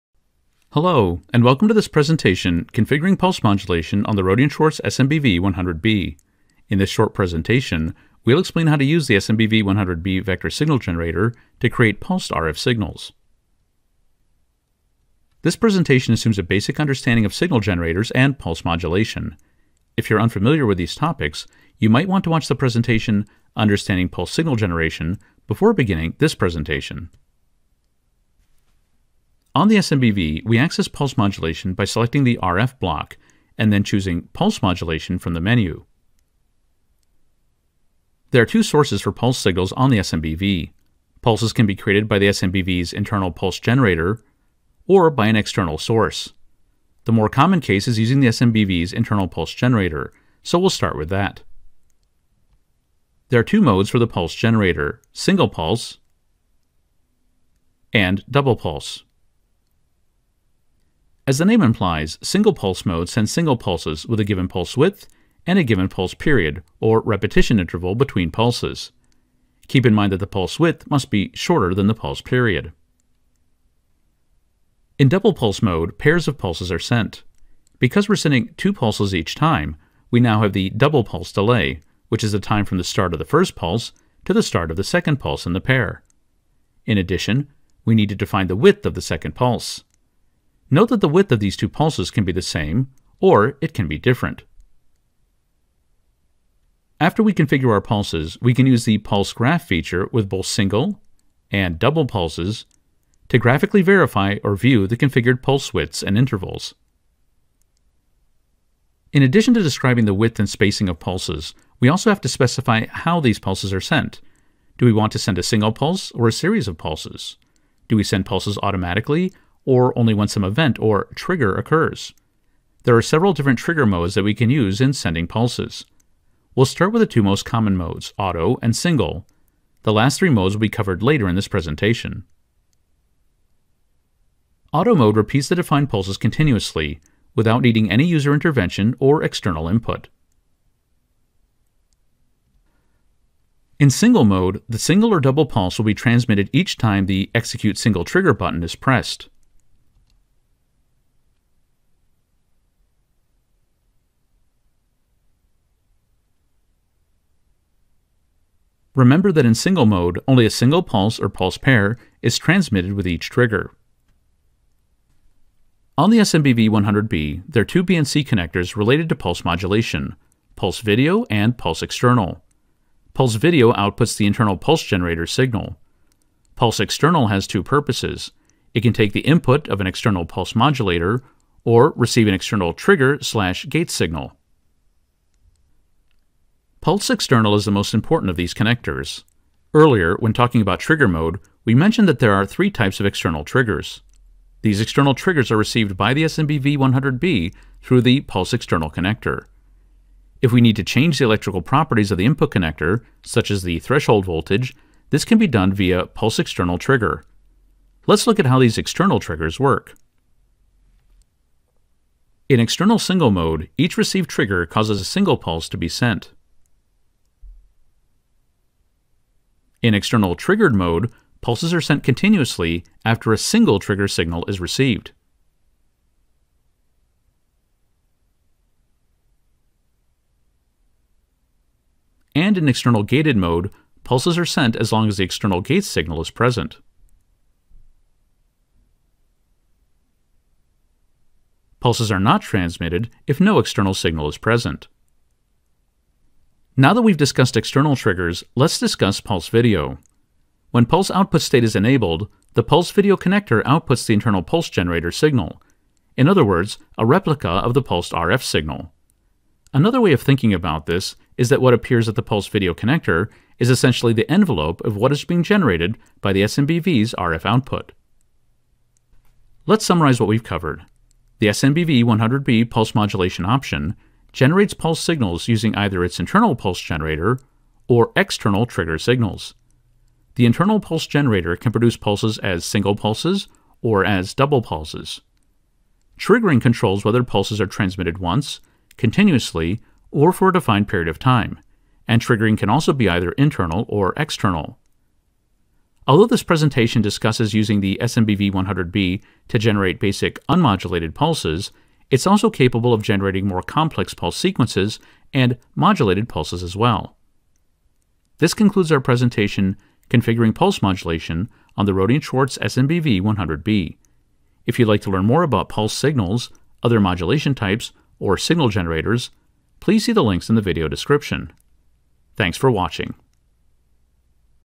Configuring Pulse Modulation on the SMBV100B
This video shows how to configure pulse modulation on a Rohde and Schwarz SMBV00B vector signal generator.

Timeline:
00:00 Introduction
00:14 Suggested viewing
00:28 Selecting pulse modulation
00:37 Pulse modulation
00:52 Pulse generator modes
01:00 Single pulse generation
01:14 Double pulse generation
01:35 Pulse graph
01:48 Trigger mode
02:13 Auto Trigger
02:22 Single trigger
02:45 Pulse modulation and external connectors
03:05 Pulse external
03:31 External pulse triggers
04:05 Pulse video
04:29 Summary